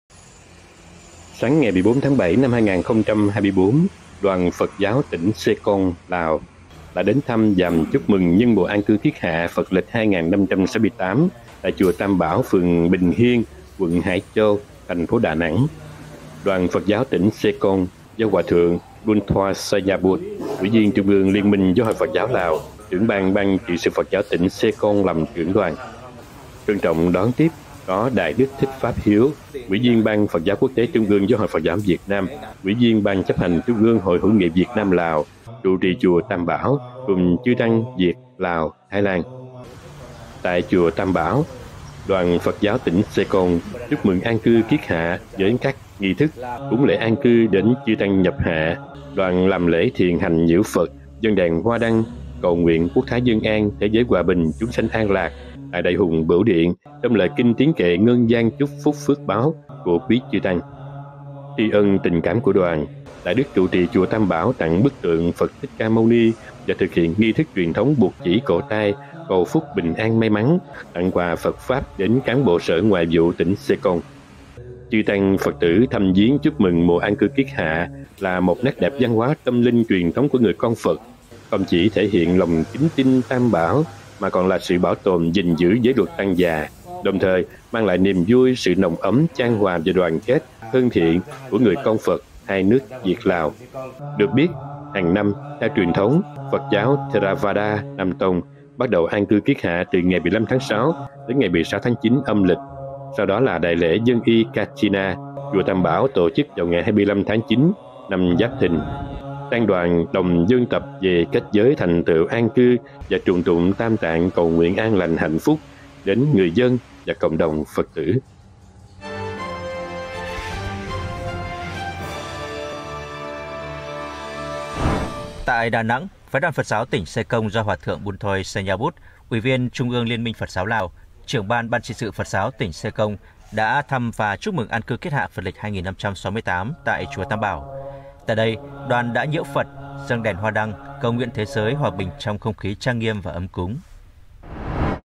Đà Nẵng- Đoàn PG Tỉnh Sekong, Lào chúc mừng An cư Kiết hạ PL.2568 tại chùa Tam Bảo
🙏Đà Nẵng- Đoàn Phật Giáo Tỉnh Sekong, Lào thăm và chúc mừng An cư Kiết hạ PL.2568 tại chùa Tam Bảo